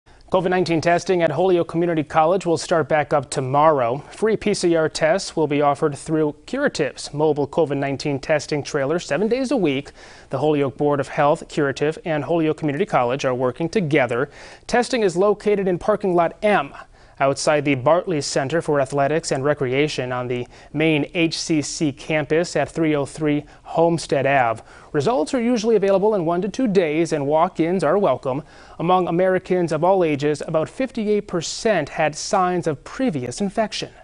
COVID-19 testing reopens at Holyoke Community College
COVID-19 testing at Holyoke Community College will start back up again on Wednesday April 27.